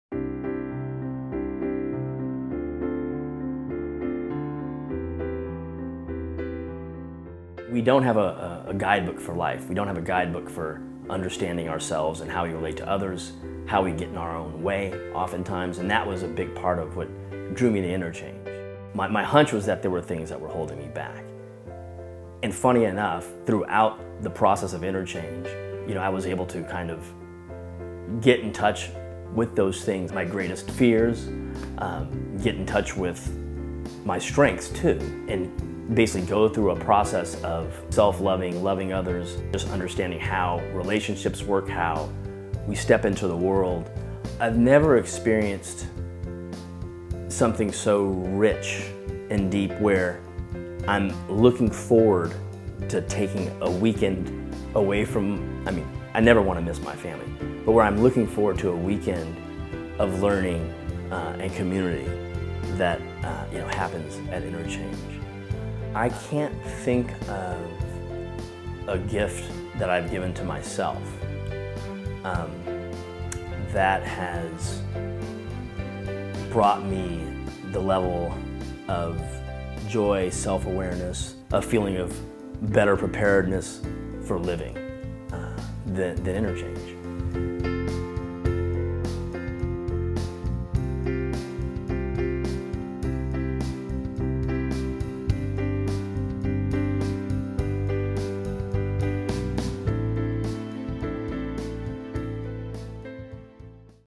James Rucker
James Rucker, 2013-14 graduate of Interchange's year-long coaching and counseling training, shares his experience of the training.